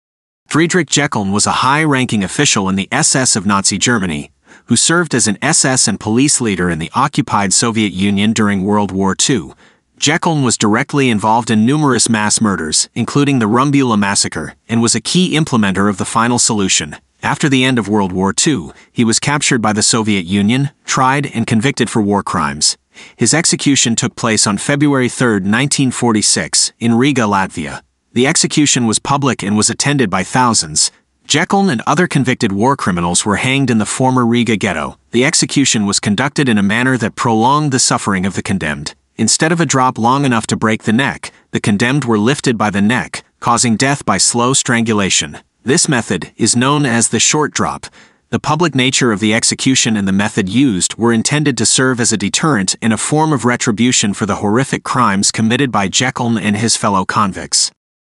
The Infamous Execution of Friedrich Jeckeln: War Crimes, Justice, and the Final Verdict
Friedrich Jeckeln was a high-ranking official in the SS of Nazi Germany who served as an SS and Police Leader in the occupied Soviet Union during World War II. Jeckeln was directly involved in numerous mass murders, including the Rumbula massacre, and was a key implementer of the Final Solution.
After the end of World War II, Jeckeln was captured by the Soviet Union, tried, and convicted for war crimes. His execution took place on February 3, 1946, in Riga, Latvia. The execution was public and was attended by thousands of people. Jeckeln and other convicted war criminals were hanged in the former Riga Ghetto.
The execution was conducted in a manner that prolonged the suffering of the condemned. Instead of a drop long enough to break the neck, which was the common method of humane hanging, the condemned were lifted by the neck, causing death by slow strangulation. This method, known as the "short drop", often resulted in a prolonged and agonizing death.
The public nature of the execution and the method used were intended to serve as a deterrent and a form of retribution for the horrific crimes committed by Jeckeln and his fellow convicts. However, the spectacle of the execution and the deliberate prolonging of suffering have been criticized as a form of "theatre of horror".

Sources:
Friedrich Jeckeln - Wikipedia
Dünamünde Action - Wikipedia
Rumbula massacre - Wikipedia
Einsatzgruppen - Wikipedia
Paul Blobel - Wikipedia
The Holocaust in Latvia - Wikipedia